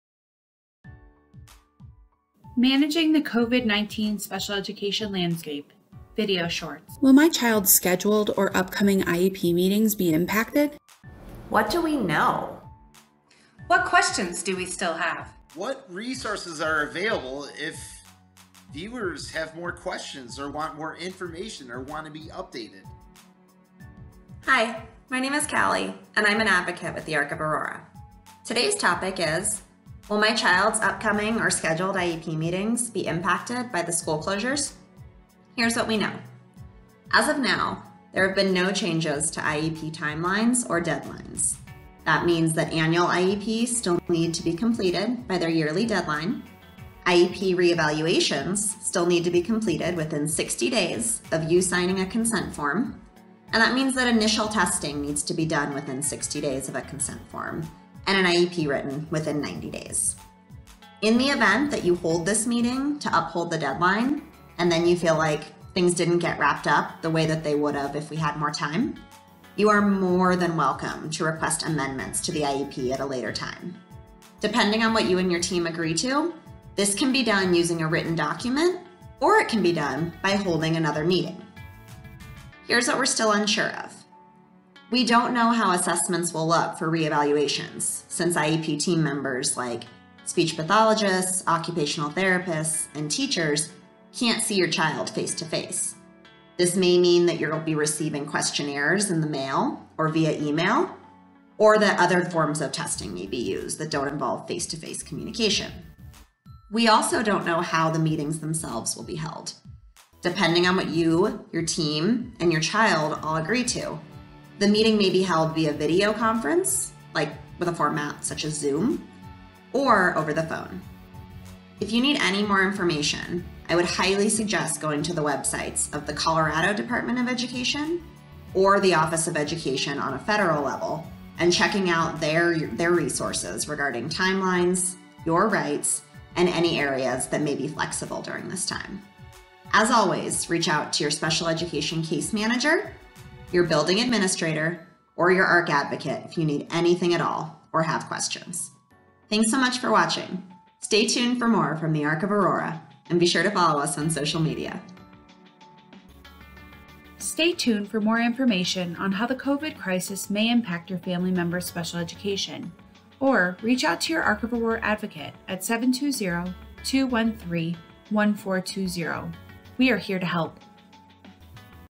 Episode 2: Managing the COVID-19 Special Education Landscape Video Shorts
Will my child’s scheduled or upcoming IEP meetings be impacted?